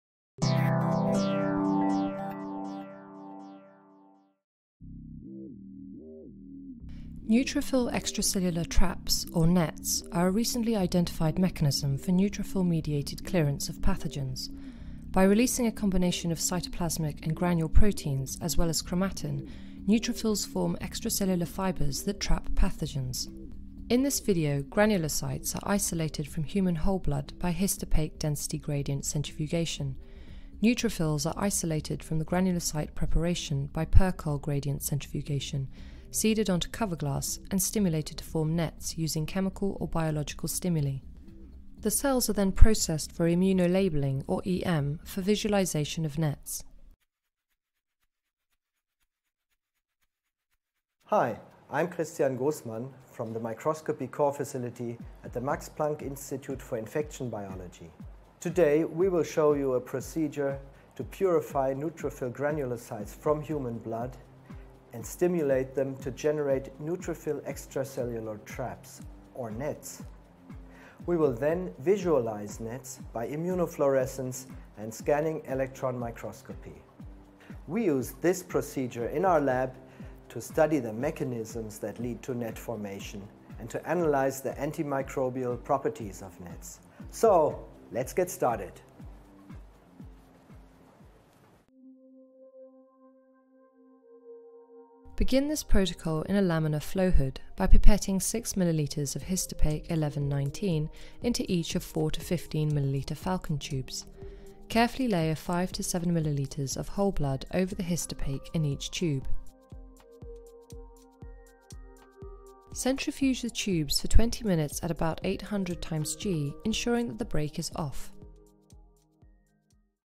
Neutrophil Extracellular Traps: Generating & Visualizing l Protocol Preview
Neutrophil Extracellular Traps: How to Generate and Visualize Them -  a 2 minute Preview of the Experimental Protocol

Volker Brinkmann, Britta Laube, Ulrike Abu Abed, Christian Goosmann, Arturo Zychlinsky
Max Planck Institute for Infection Biology, Core Facility Microscopy; Max Planck Institute for Infection Biology, Cellular Microbiology;

Neutrophil Extracellular Traps (NETs) are an important innate immune mechanism to fight pathogenic bacteria, fungi and parasites. Here we describe methods to isolate neutrophil granulocytes from human blood and to activate them to form NETs. We present preparation techniques to visualize NETs in light and electron microscopy.